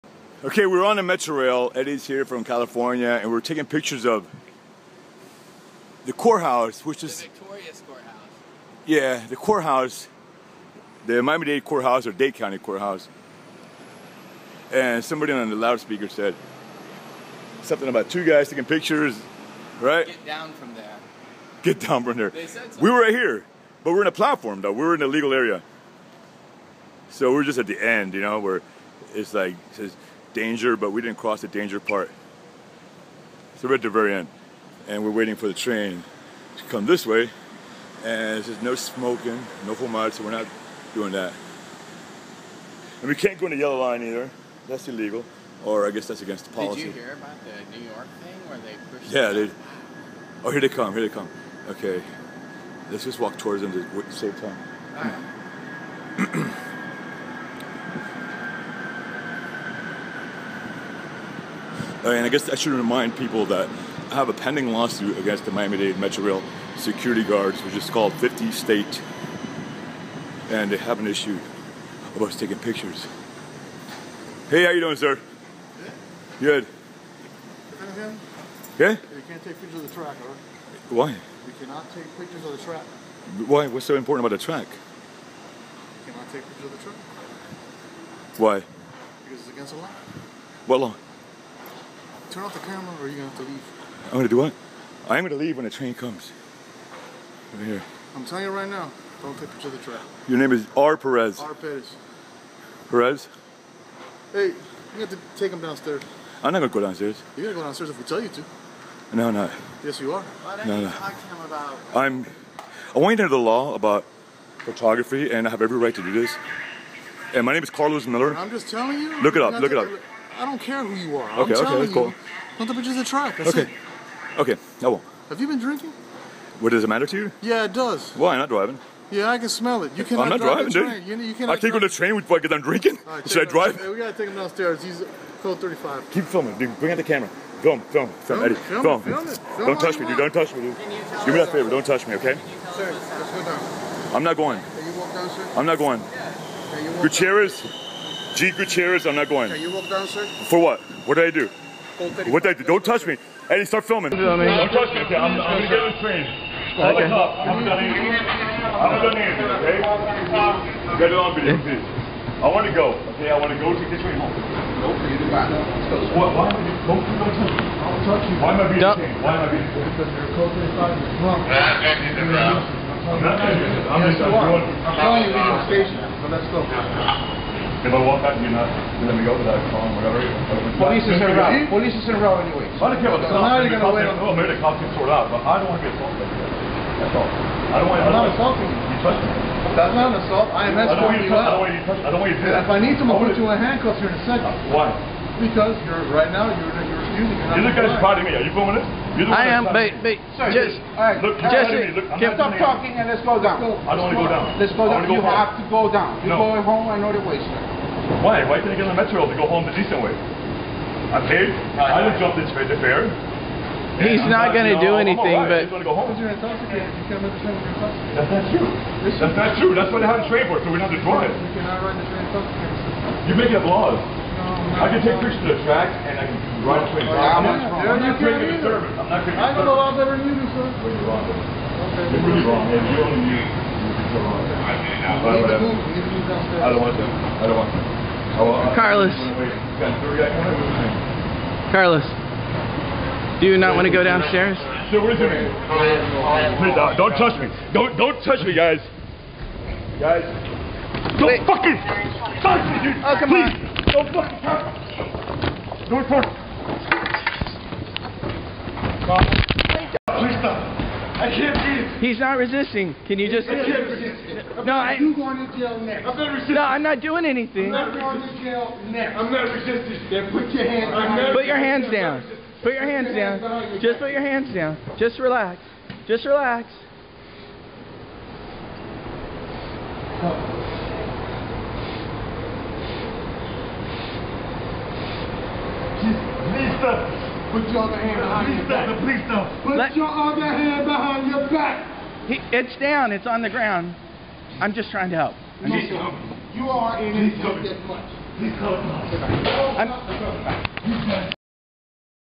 50 State Security guards attack Carlos Miller for taking photos off Miami-Dade Metrorail platform
The name of the company is 50 State Security. It wasn't 50 security guards that attacked me.